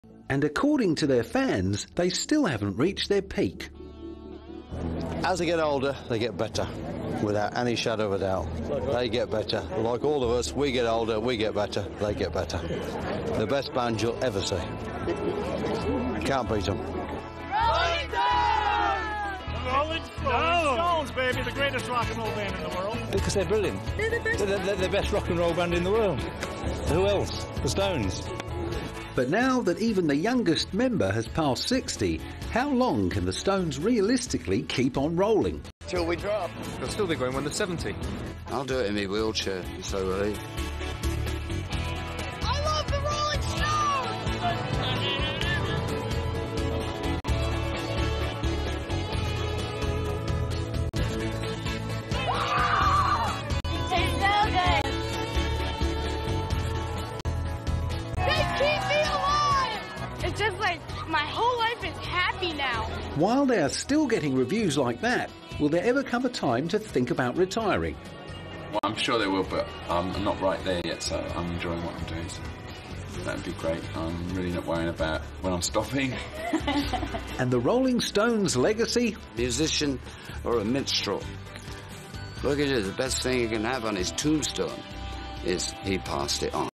The Rolling Stones How Long Can the Legends Keep on Rolling?
The video effectively captures the viewer's attention with an intriguing hook about the Rolling Stones' continued success. It maintains coherence throughout, building a clear narrative on the band's legacy. The completeness of the video is commendable, providing a thorough discussion. The personal connection and trend topic aspects add value to the video. Improvement could be made with a call to action for viewers.